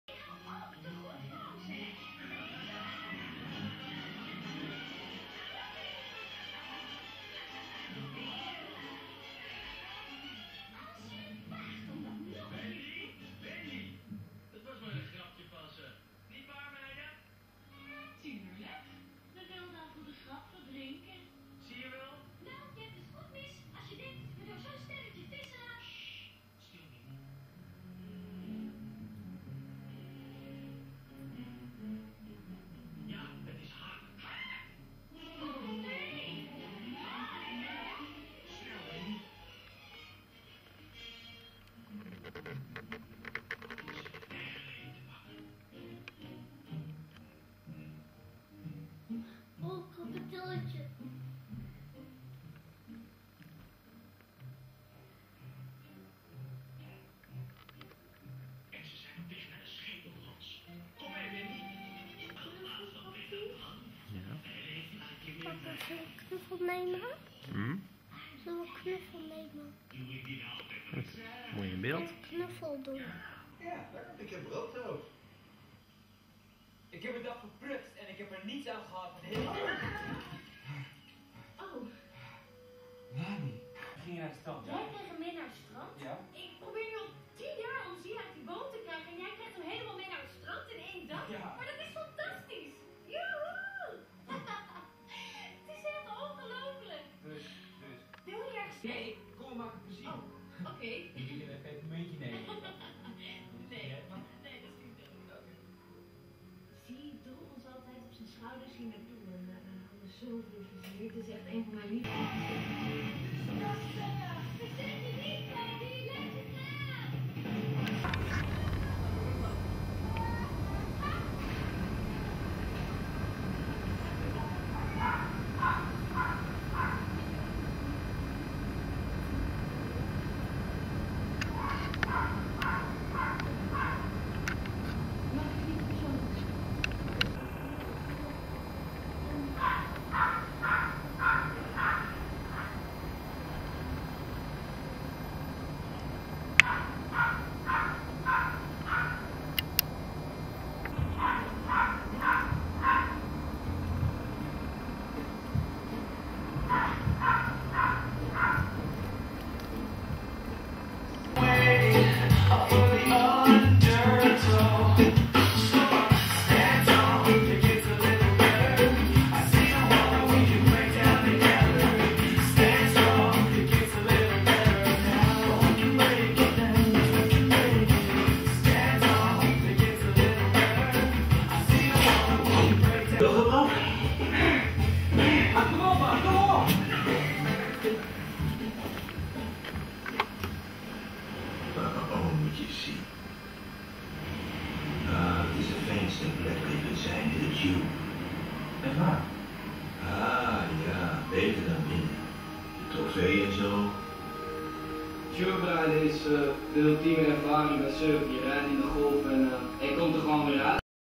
HD video test
Dit is een videotest met een Sony PRO HD camera
Ochtend, Yourick tv kijkend op de bank